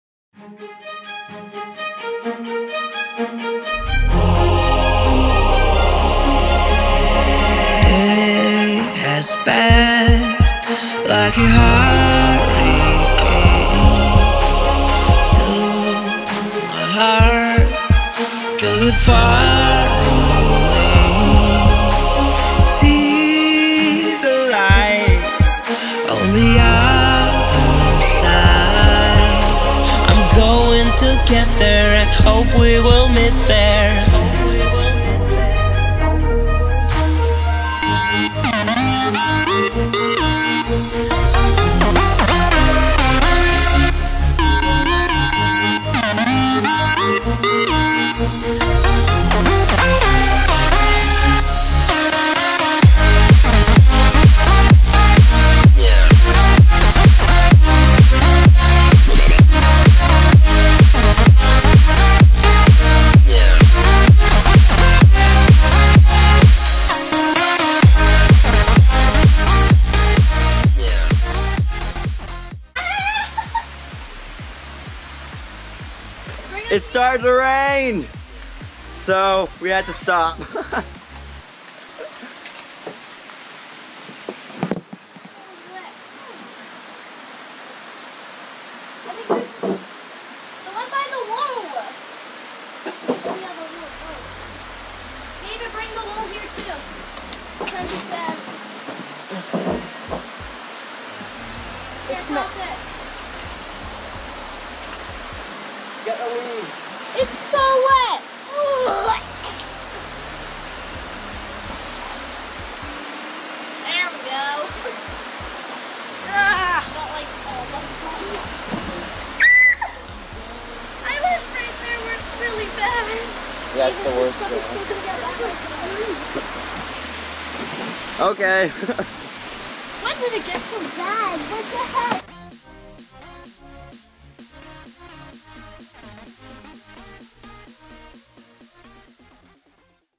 Timelapse - Forest Hut
Today We have a Time lapse in honour of 1 year on YouTube :D

The Music that I have used in this Video can be found just below:

This Video Was Inspired By:
Primitive Technology